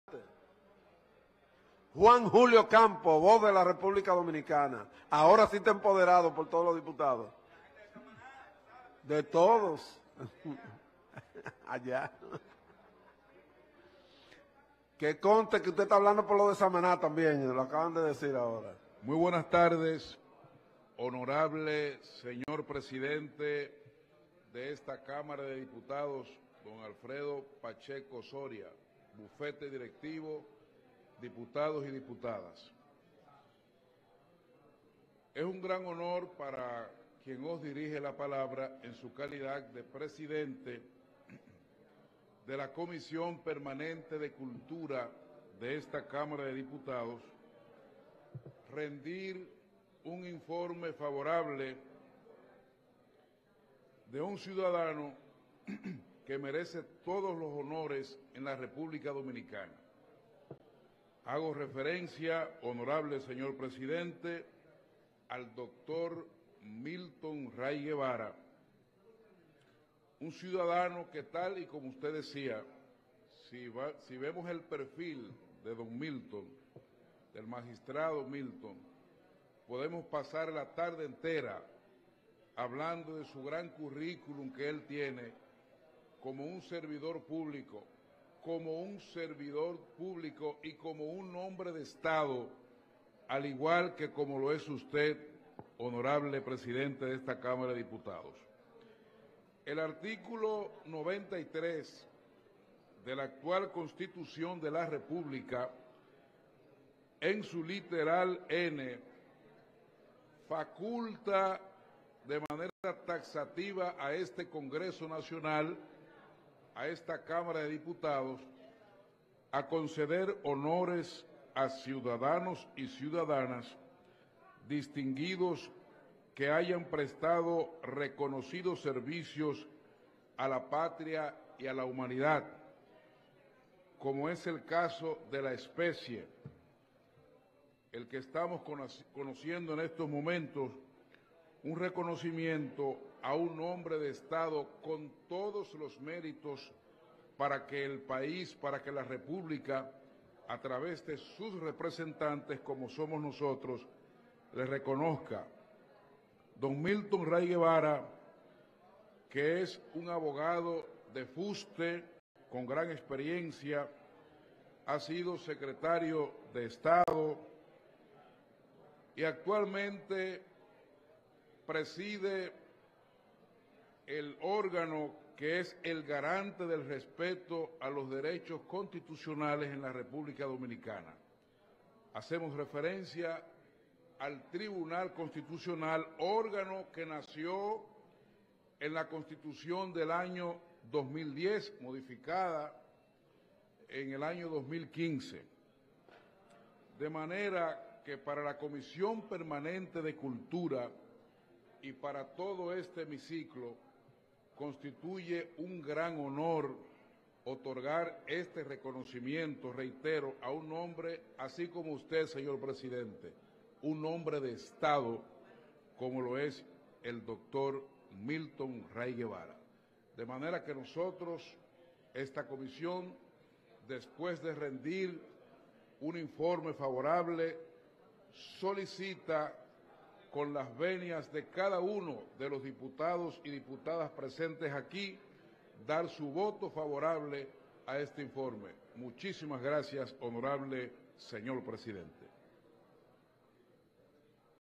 Turno del diputado Juan Julio Campos, Sesión No. 0010 miércoles 04 de octubre 2023.
El diputado @juancamposv como presidente de la Comisión de Cultura, sostiene que el informe rendido por dicha Comisión ha sido favorable. Expresa que ha sido un honor trabajar enel proyecto de resolución que reconoce al doctor Milton Ray Guevara, ya que él, es un ciudadano que merece todos los honores del país; explica que tiene un valioso currículum como Servidor Público y hombre de estado.

Expresa que el Magistrado ya mencionado ha logrado destacarse en cada área del estado en la que ha ocupado; dice que en la actualidad es el presidente del Tribunal Constitucional del país, donde desempeña una labor extraordinaria. Solicita el voto favorable.